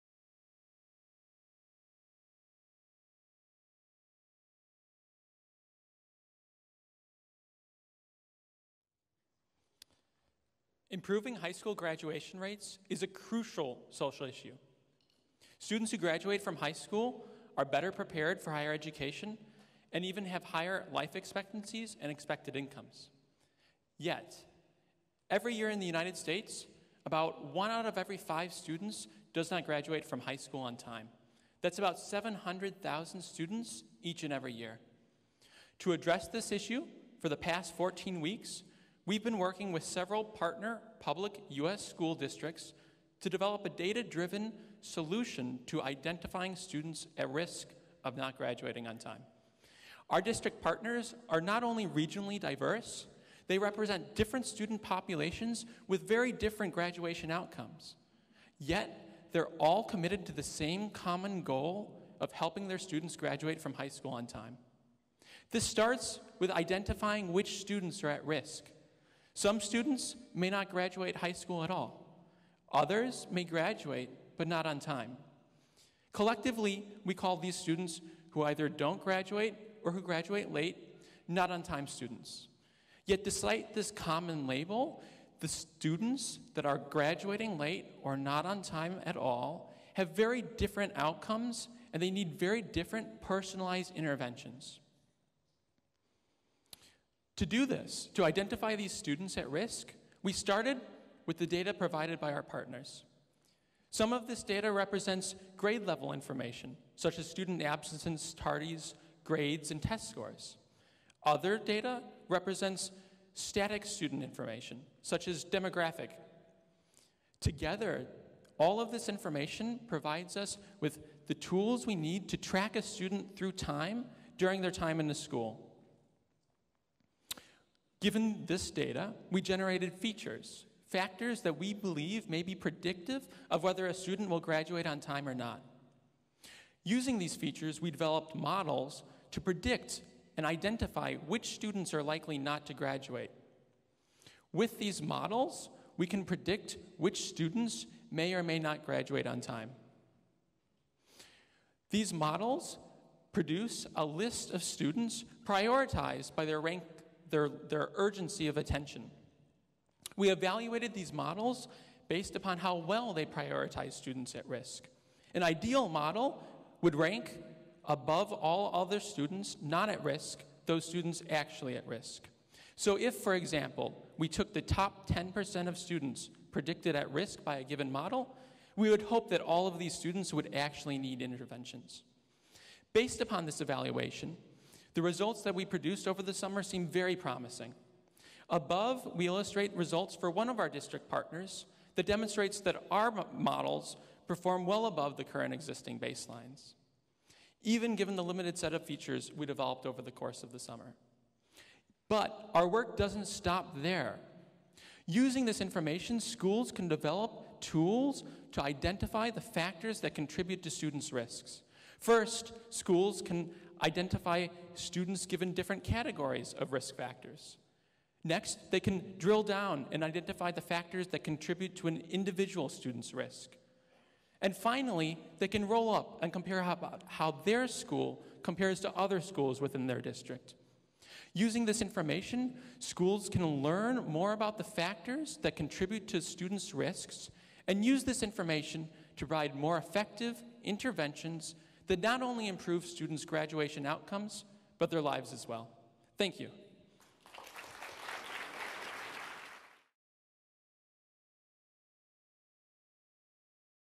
2015 DSSG Data Fest: Team High School Graduation
"Increasing On-Time High School Graduation Rates" - a presentation from the 2015 Data Science for Social Good Data Fest, August 26, 2015.

Speaker: Reid Johnson

Team: Kerstin Frailey, Robin Gong, Siobhan Greatorex-Voith, Reid Johnson, Anushka Anand, Alan Fritzler

Partner: Wake County Public School System, Arlington Public Schools, Vancouver Public Schools

For more information on Data Science for Social Good, visit dssg.uchicago.edu